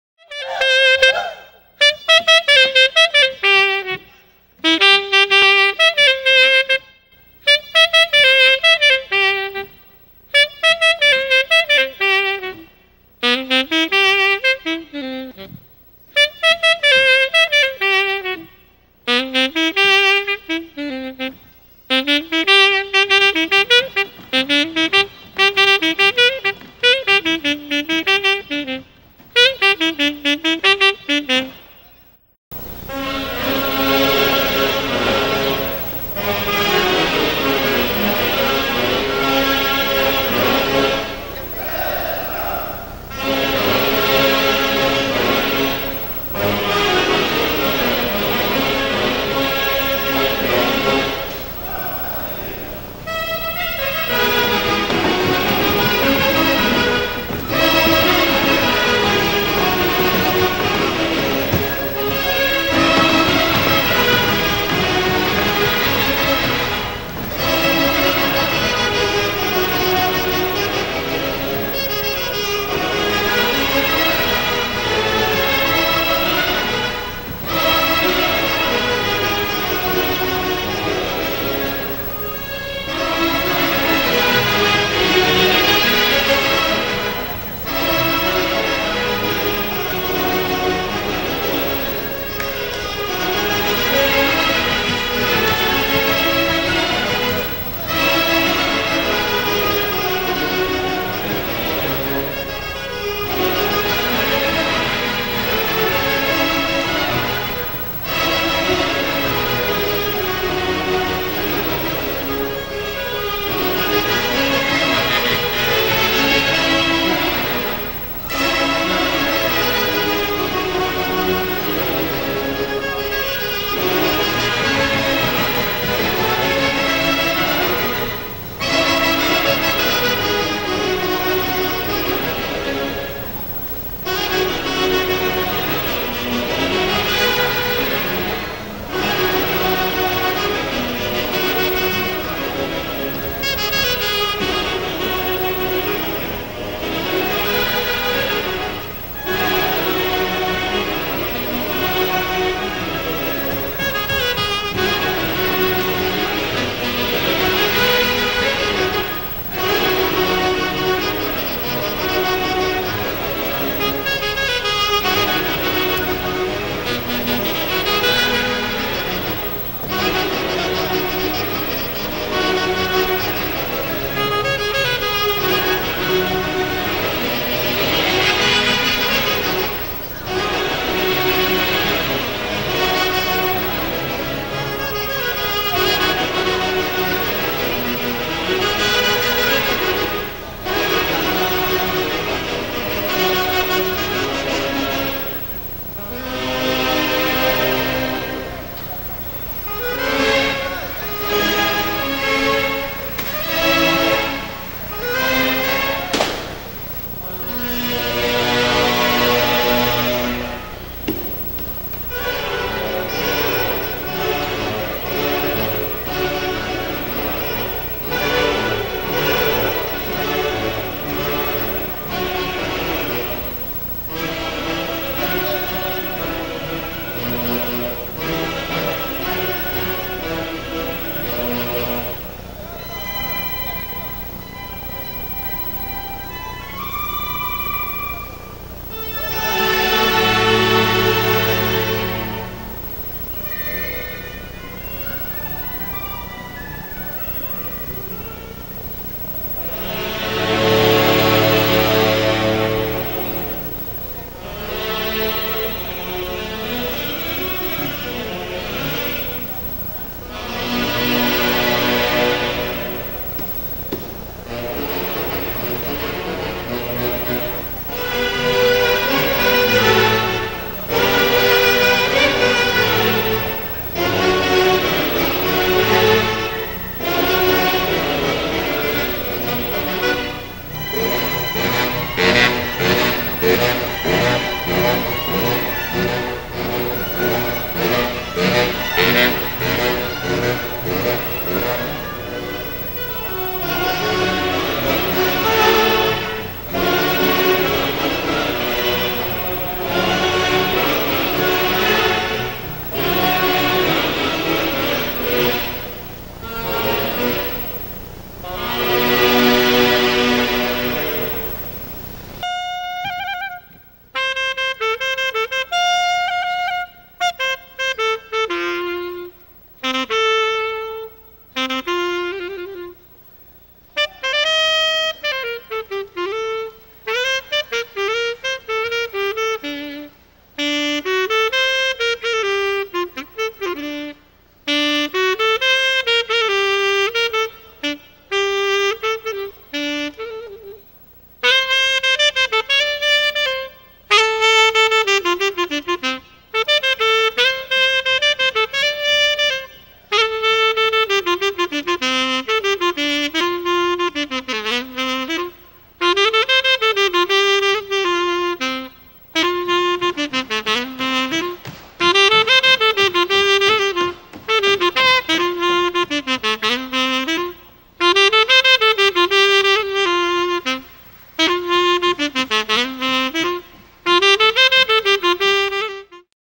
ORQUESTA REVELACIÓN HUANCAINA 2008 -
TUKILLA PERU PRESENTA EL GRAN CONCURSO DE ORUESTAS TIPICAS  FRAMA 2008 - REALIZADO EN EL GRAN PARQUE DE EXPOSICIÓN LIMA - PERU - AÑO 2008